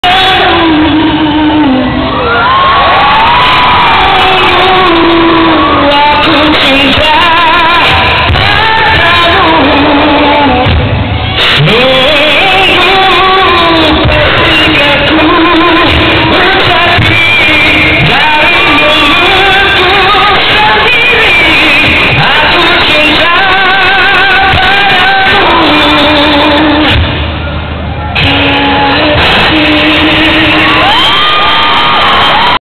Malay song
gary at 1u countdown singing a malay song..
i gotten the title